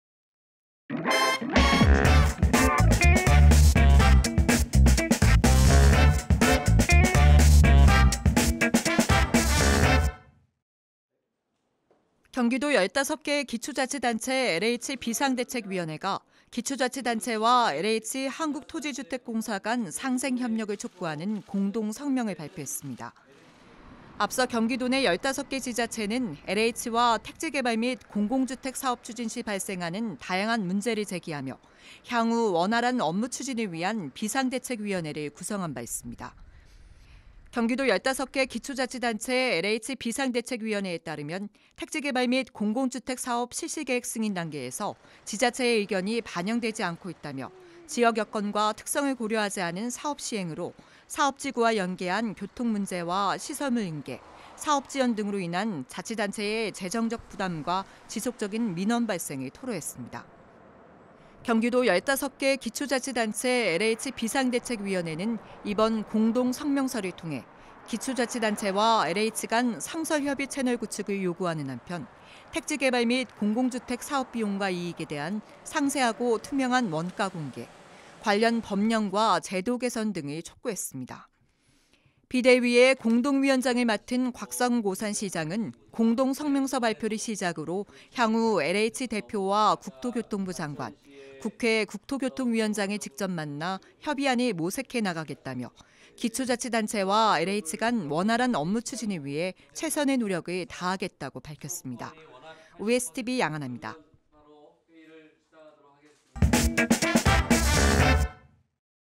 경기도 15개 지자체 ‘공동성명’...“LH-지자체 협력” 촉구
경기도 15개 기초자치단체 LH비상대책위원회가 13일 기초자치단체와 LH 한국토지주택공사 간 상생협력을 촉구하는 공동성명을 발표했습니다.

앞서 경기도 내 15개 지자체는 LH와 택지개발 및 공공주택사업 추진 시 발생하는 다양한 문제를 제기하며, 향후 원활한 업무추진을 위한 비상대책위원회를 구성한 바 있습니다.

경기도 15개 기초자치단체 LH비상대책위원회에 따르면 “택지개발 및 공공주택사업 실시계획 승인 단계에서 지자체의 의견이 반영되지 않고 있다”며, “지역 여건과 특성을 고려하지 않은 사업 시행으로, 사업지구와 연계된 교통 문제와 시설물 인계·인수, 사업 지연 등으로 인한 자치단체의 재정적 부담과 지속적인 민원 발생”을 토로했습니다.

경기도 15개 기초자치단체 LH비상대책위원회는 이번 공동성명서를 통해 기초자치단체와 LH간 상설 협의 채널 구축을 요구하는 한편, 택지개발 및 공공주택 사업 비용과 이익에 대한 상세하고 투명한 원가 공개, 관련 법령과 제도 개선 등을 촉구했습니다.

비대위의 공동위원장을 맡은 곽상욱 오산시장은 “공동성명서 발표를 시작으로, 향후 LH 대표와 국토교통부 장관, 국회 국토교통위원장을 직접 만나 협의안을 모색해 나가겠다.”며, “기초자치단체와 LH 간 원활한 업무추진을 위해 최선의 노력을 다하겠다.“고 밝혔습니다.